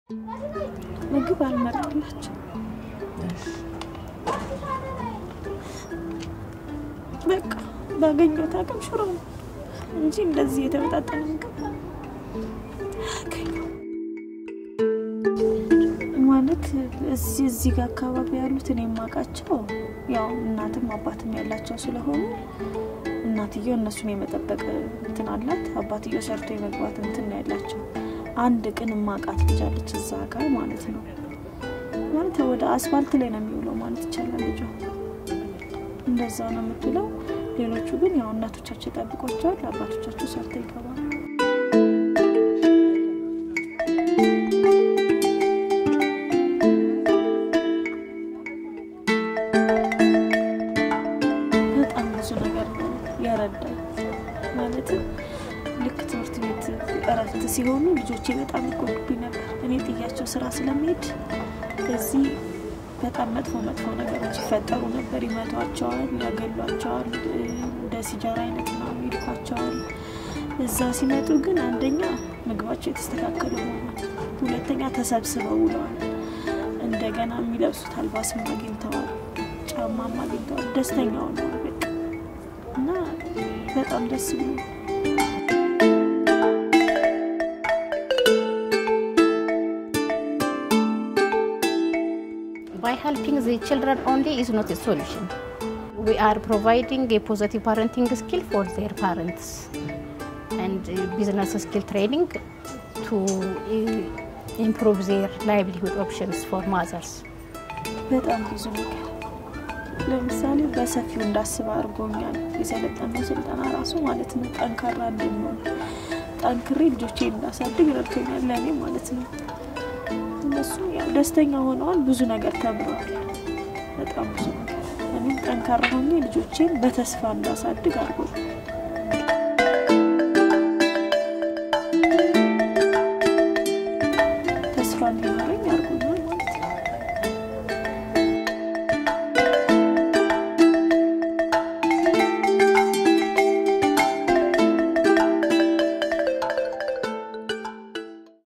A single mother’s determination to protect her sons | Ethiopia
Etalem is a single mother fighting the odds to raise her two sons, Aaron*,7, and Amadi*, 5, in a slum in Ethiopia. Their father left when Etalem was pregnant with Amadi. Etalem grew up without parental care. She has no family to support her provide for her children or to watch them when she is at work. She is trying her best to be a good mother to her sons, while struggling to provide for their basic needs and keep them off the streets.

Listen to their story.

Video: Petterick Wiggers